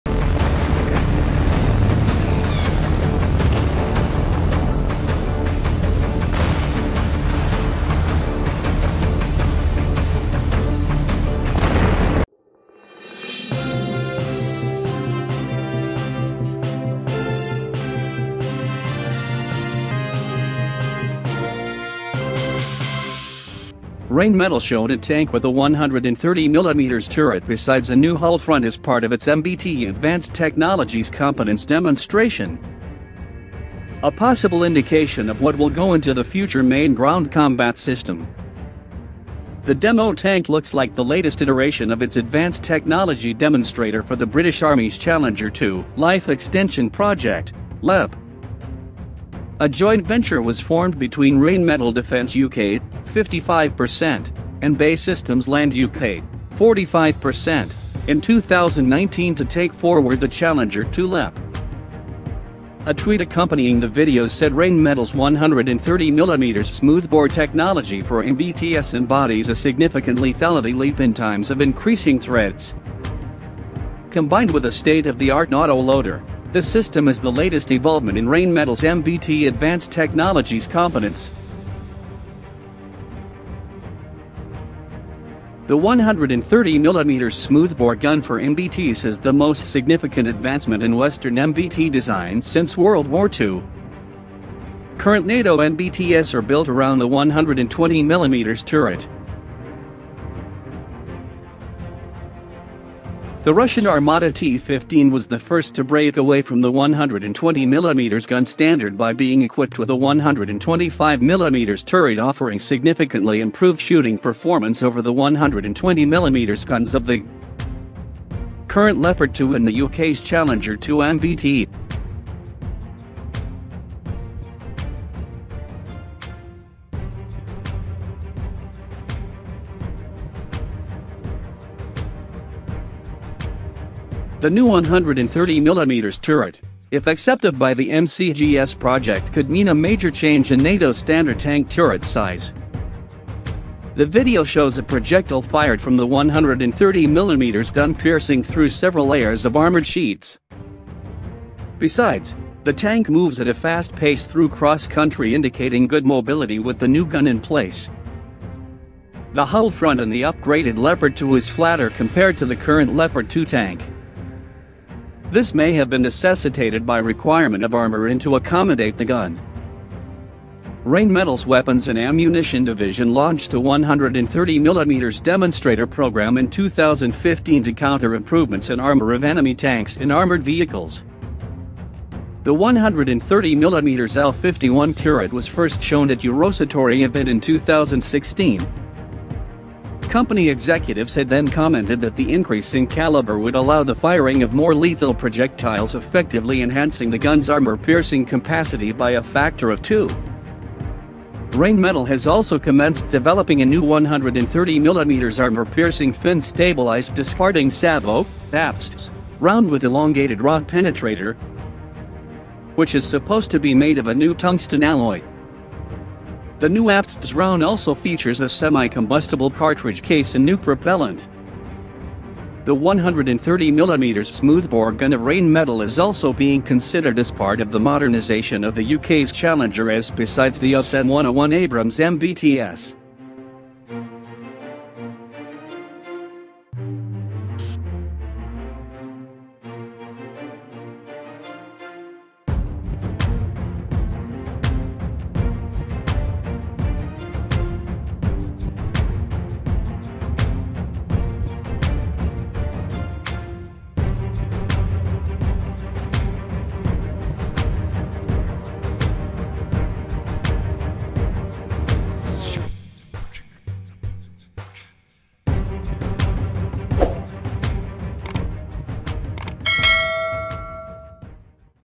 New From Germany : New Turret equipped with Rheinmetall 130mm smoothbore gun.
130mm smoothbore technology for MBTs embodies a significant lethality leap in times of increasing threats. Combined with a state-of-the-art auto-loader, this system is the latest evolvement in Rheinmetall’s MBT Advanced Technologies
The 130mm smoothbore gun for MBT’s is the most significant advancement in Western MBT design since World War II. Current NATO MBTs are built around the 120mm turret.

The Russian Armata T-15 was the first to break away from the 120mm gun standard by being equipped with a 125mm turret offering significantly improved shooting performance over the 120mm guns of the current Leopard 2 and the UK’s Challenger 2 MBT.